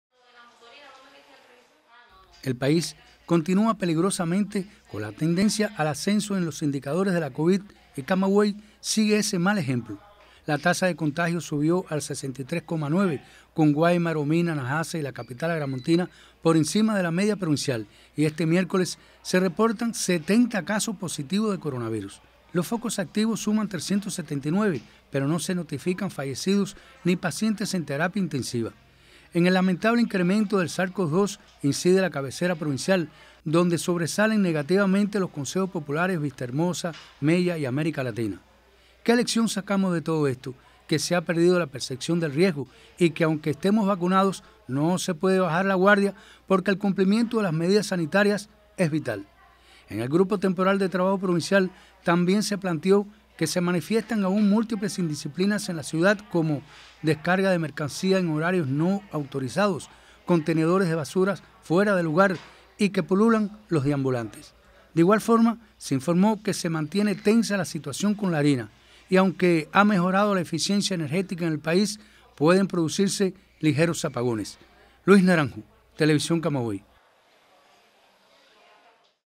Aumenta en Camagüey nuevos casos positivos de Covid-19
Cuba confirma 811 casos positivos a la Covid-19 y ningún fallecidos a causa de esta enfermedad. De la cifra de nuevos contagiados, 70 corresponde a la provincia de Camagüey. El aumento de personas enfermas por el coronavirus en las últimas jornadas, confirma que se ha pedido la percepción del riesgo en la población.